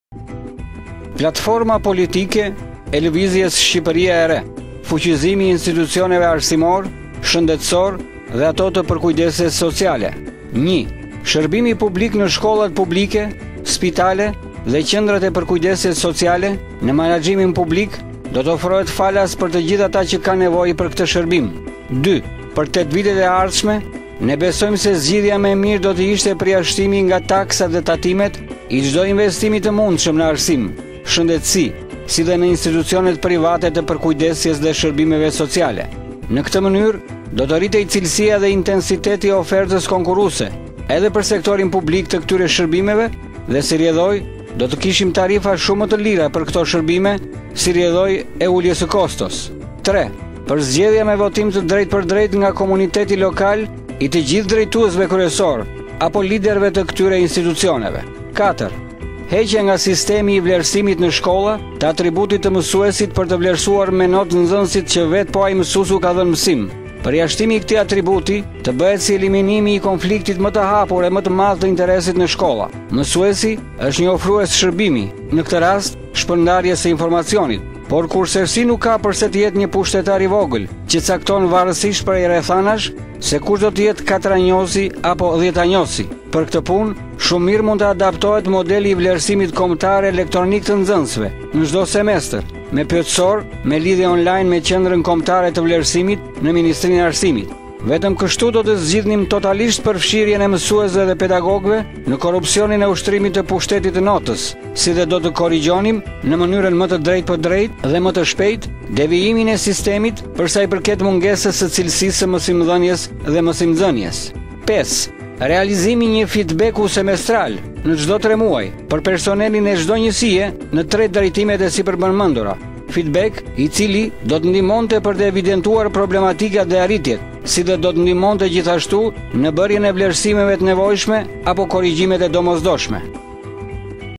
PIKA E DYTE E PLATFORMES POLITIKE TE LSHR-së
Fuqizimi i institucioneve Arsimore  Shëndetësore dhe ato të Përkujdesjes Sociale

1 -- Shërbimi publik në shkollat publike , spitale dhe qendrat e përkujdesjes sociale në menaxhimin publik  do të ofrohet falas për të gjithë ata që kanë nevojë për këtë shërbim.

2 -- Për 8 vitet e ardhshme ne besojmë se zgjidhja më e mirë do të ishte përjashtimi nga taksat dhe tatimet i çdo investimi të mundshëm në Arsim  , Shëndetësi  dhe në institucionet private të  Përkujdesjes  dhe  Shërbimeve Sociale. Në këtë mënyrë do të rritej cilësia dhe intensiteti i ofertës konkurruese  edhe për sektorin publik të këtyre shërbimeve dhe si rrjedhojë do të kishim tarifa shumë më të lira për këto shërbime si rrjedhojë e uljes së kostos.

3 -- Përzgjedhja me votim të drejtpërdrejtë nga komuniteti lokal i të gjithë drejtuesve kryesor apo liderëve të këtyre  institucioneve.

4 -- Heqja nga sistemi i vlerësimit në shkolla të atributit të mësuesit për të vlerësuar me notë nxënësit që vet po ai mësues u ka dhënë mësim. Përjashtimi i këtij atributi  të bëhet si  eliminimi i konfliktit më të hapur dhe më të madh të interesit në shkolla. Mësuesi është një ofrues shërbimi, në këtë rast , shpërndarjes së informacionit, por kurrsesi nuk ka përse të jetë një pushtetar i vogël që cakton varësisht prej rrethanash, se kush do të jetë ''katranjosi'' apo ''dhjetanjosi''. Për këtë punë, shumë mirë mund të adaptohet modeli i vlerësimit kombëtar elektronik të nxënësve në çdo semester (me pyetësor, me lidhje online me Qendrën Kombëtare të Vlerësimit në Ministrinë e Arsimit). Vetëm kështu do të zgjidhnim totalisht përfshirjen e mësuesve dhe pedagogëve në korrupsionin e ushtrimit të pushtetit të notës si dhe do të korrigjonim në mënyrë më të drejtpërdrejtë  dhe më të shpejtë  devijimin e sistemit përsa i përket mungesës së cilësisë së mësimdhënies dhe mësimnxënies.

5 -- Realizimi i një feedback semestral (çdo tre muaj) për personelin e çdo njesie në tre drejtimet e sipërpërmendura, feedback i cili do të na ndihmonte për të evidentuar problematikat dhe arritjet si dhe do të ndihmonte në bërjen e vlerësimeve te nevojshme apo korrigjimet e domosdoshme.